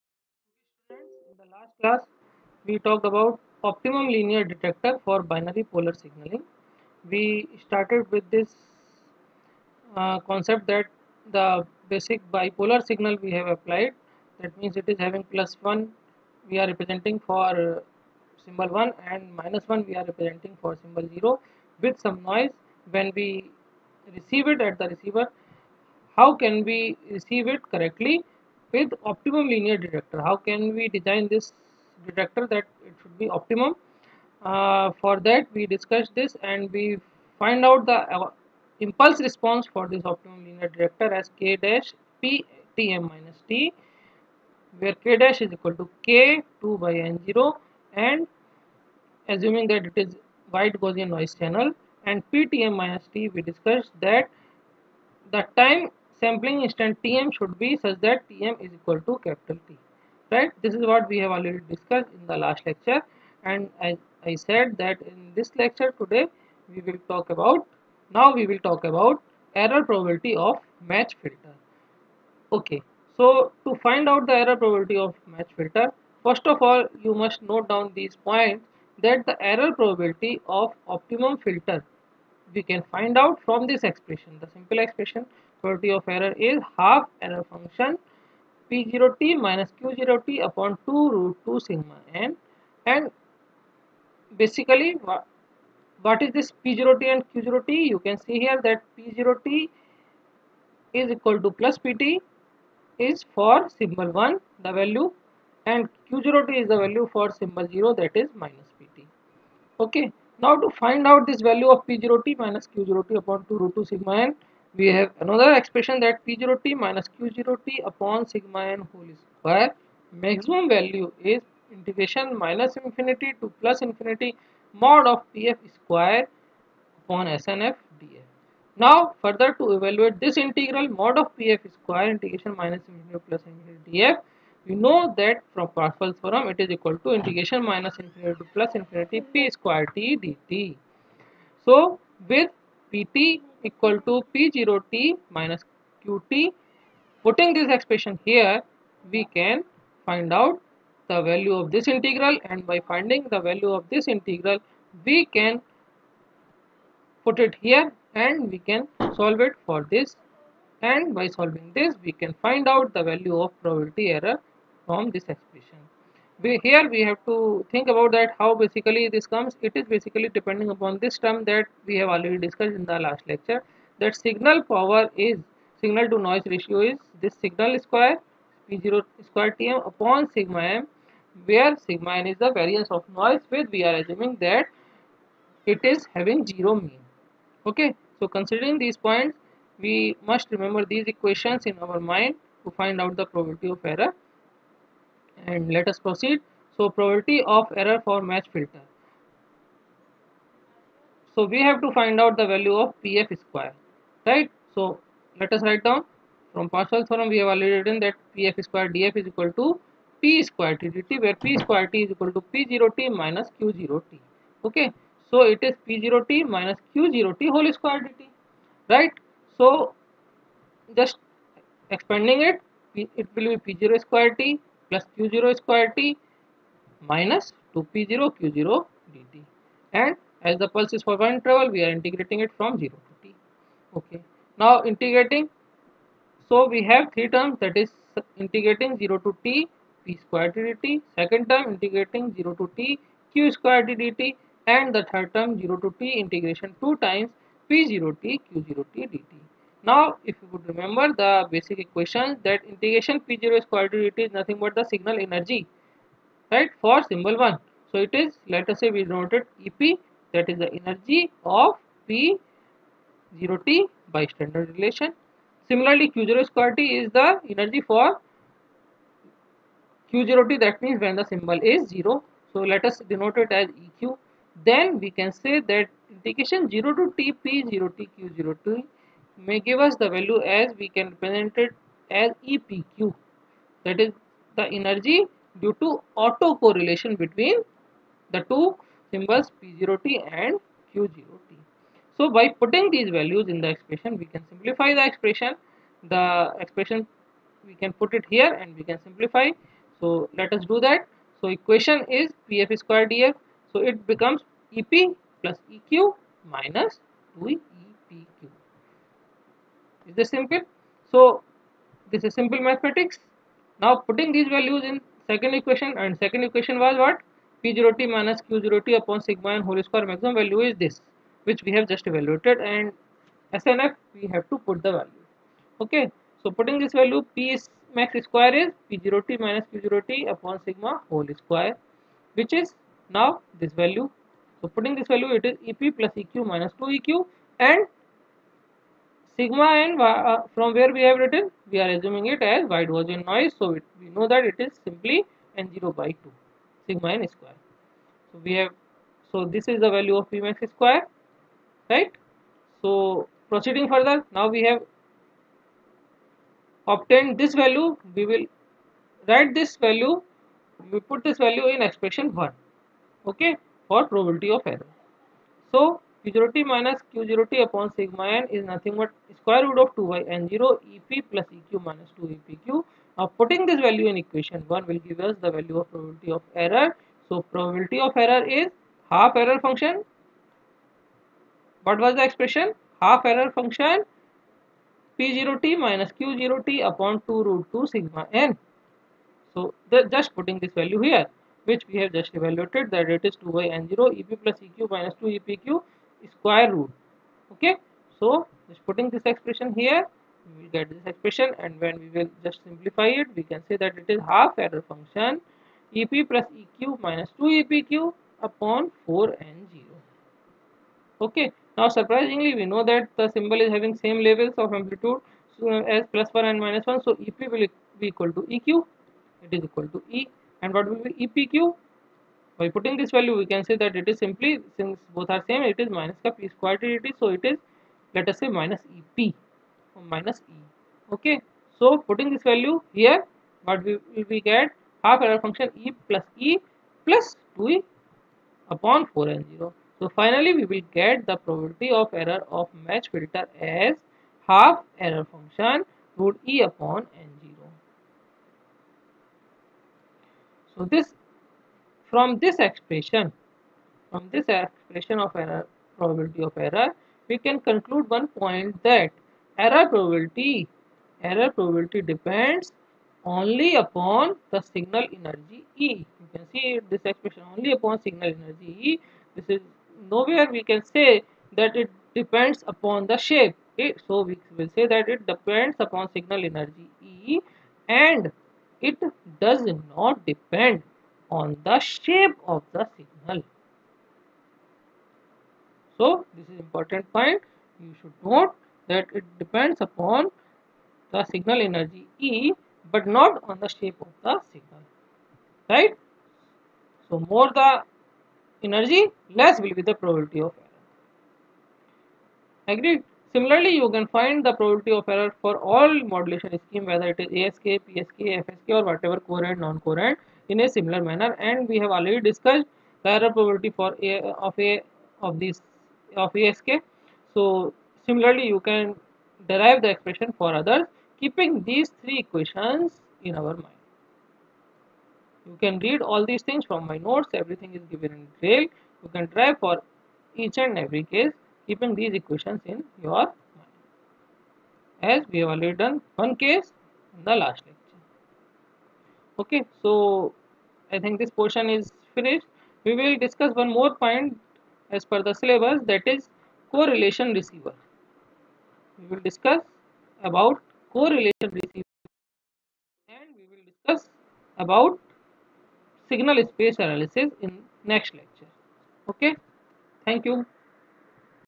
DC Unit3 L3 Error probability of matched Filter by Mr. Shailendra Bisariya
This video belongs to the syllabus of unit 3 for subject Digital Communication as prescribed by Abdul Kalam Technical University (AKTU), Lucknow.

As per the syllabus of unit 3, the topics required to cover are : Performance Analysis of Digital communication system: Optimum linear Detector for Binary polar signaling, General Binary Signaling, Coherent Receivers for Digital Carrier Modulations, Signal Space Analysis of Optimum Detection,
Vector Decomposition of White Noise Random processes and General Expression for Error Probability of optimum receivers.

In the series of video lectures this video covers about Error probability calculation specially error probability of matched Filter.